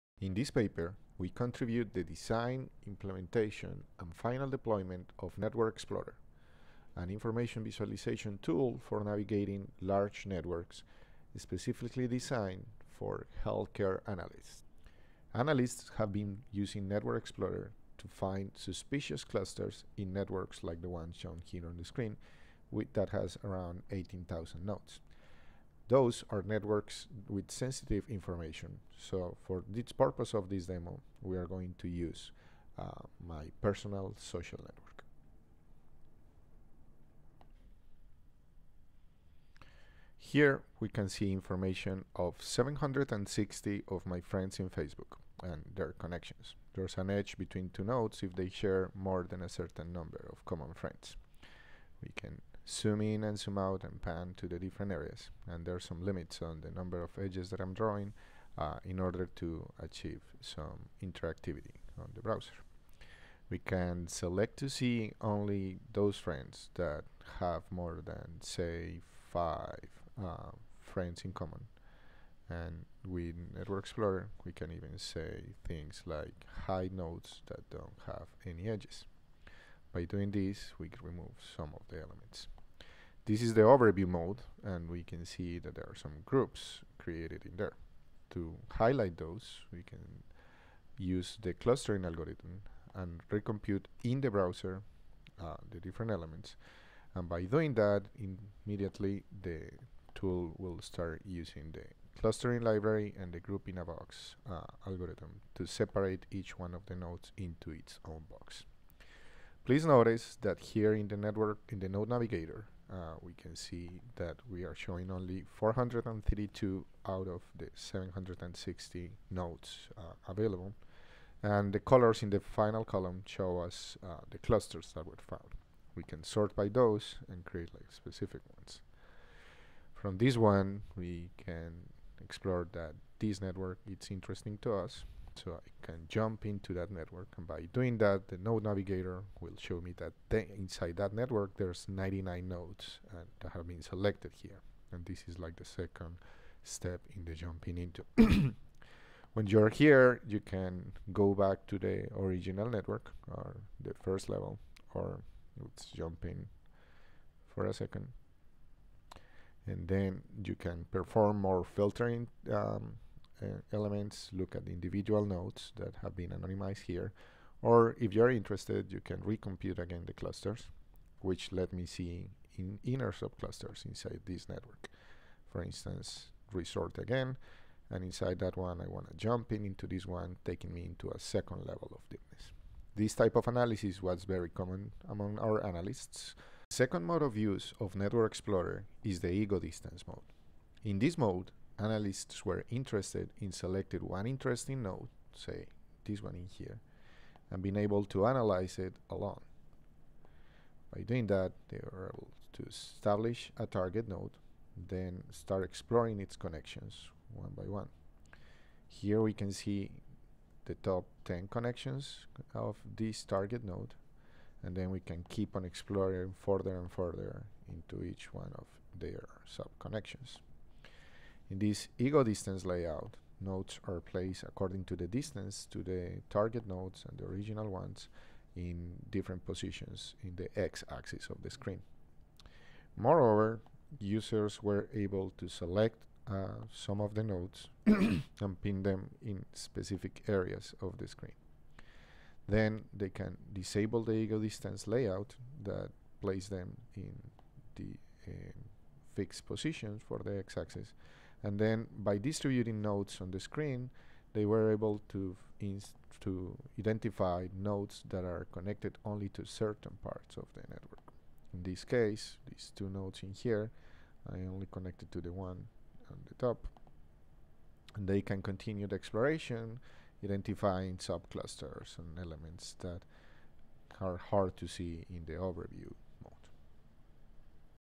NetworkExplorer AVI Demo
This paper describes the process of design, implementation, and real world deployment of a web-based network exploration tool called Network Explorer. We designed Network Explorer based on the expressed needs of our clients and later deployed it as part of a larger system for fraud detection in health care. Our implementation of Network Explorer provides visual interactive access to large-scale net- work data. As part of the Network Explorer tool we contribute a dynamic group-in-a-box implementation for lay- ing out clusters, and a node navigator widget that aids in the exploration of large networks. We are also contributing two open source components of the Network Explorer for the community to reuse: an in-browser clustering library, and the dynamic group-in-a-box algorithm. We have evaluated the network explorer tool in multiple real-world environments including the fraud detection setting above.